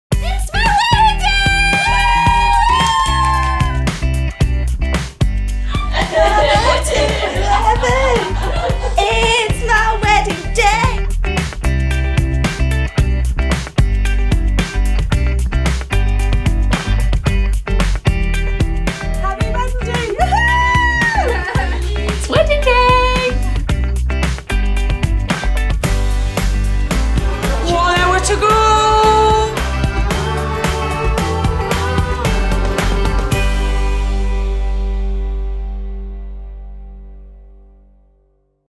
Morning of the Wedding - Bridal Party!
See what our Brides get up to on the morning of their big day!